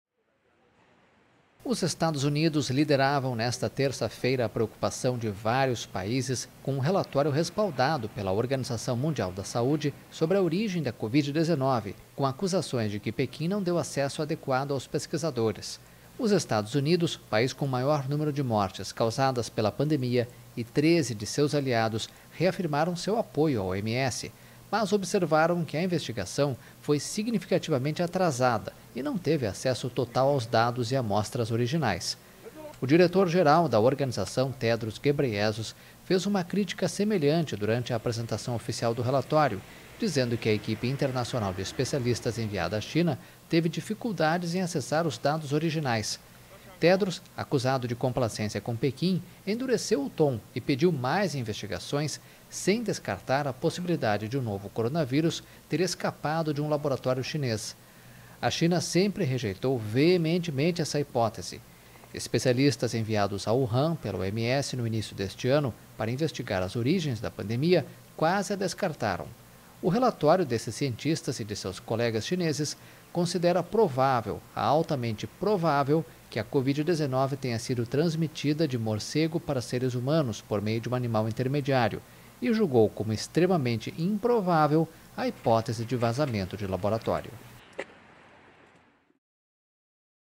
Preocupação com o relatório da OMS | AFP
Vários países, entre eles os Estados Unidos, manifestaram preocupação com um relatório respaldado pela Organização Mundial da Saúde sobre a origem da covid-19. of WHO experts' Wuhan mission